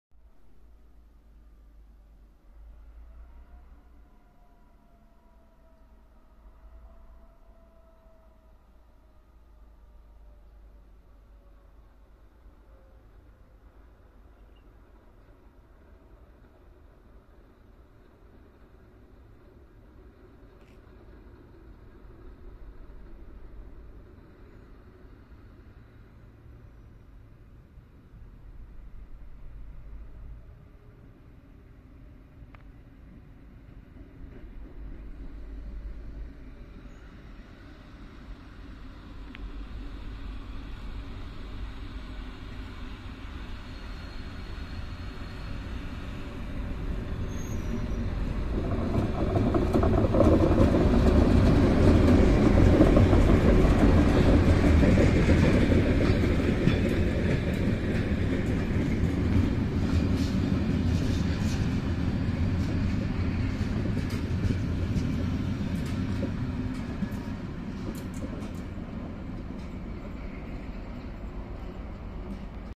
2022-11-27 Ludmilla mit 2Salonwagen durch Hennersdorf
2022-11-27 Ludmilla mit 2Salonwagen durch Hennersdorf
Spontaner Besuch der Ludmilla am Abend und bei eisigen Temperaturen.
Die Waggons wurden in Schlettau geholt und die Reise ging weiter nach Chemnitz.